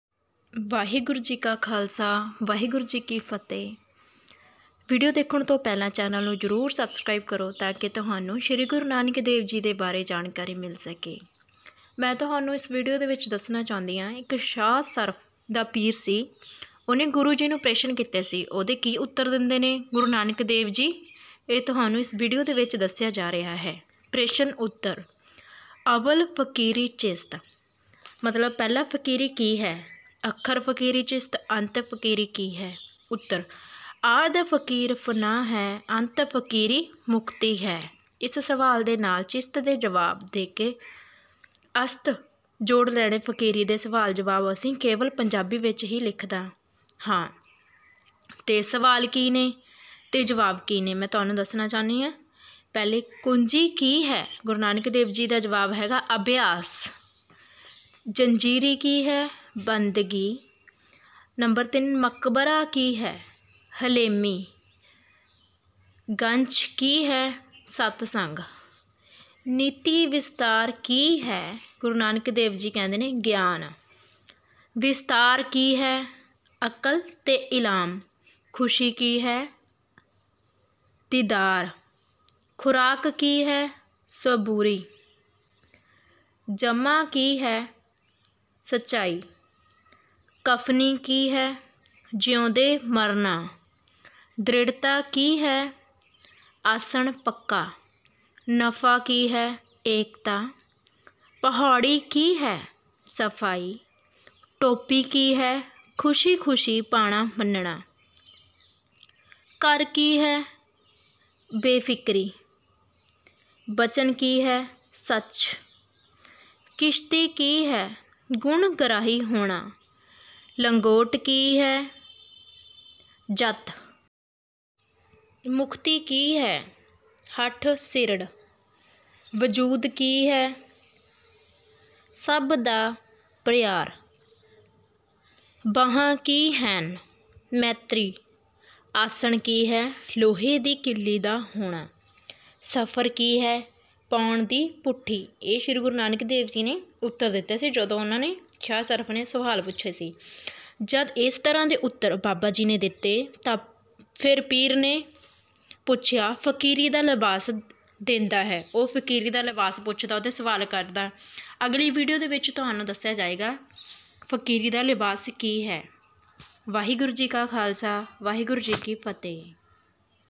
( ਸਾਹ ਸ਼ਰਫ ਦੁਆਰਾ ਪੁਛੇ ਗਏ ਸਵਾਲ ਤੇ ਅੱਗੋ ਸ਼੍ਰੀ ਗੁਰੂ ਨਾਨਕ ਦੇਵ ਜੀ ਨੇ ਕੀ ਜਵਾਬ ਦਿੱਤਾ )
sat shri akal ji es video vich shri guru nanak dev ji ki jwab dinde hen o ea video vich dsea gya he ji shri guru nanak dev ji jdo shah saraf guru ji nu swal krda he ta guru ji ne apne sbda ch usnu es tra de jwab dite ta aggo o fr swal krda he o next video vich dikhaya jaye ga video wdia lge ta ea channel nu jrur subscribe kro ta ke thnu sbnu guru ji bare ds skiye waheguru ji ka khalsa waheguru ji ki fathe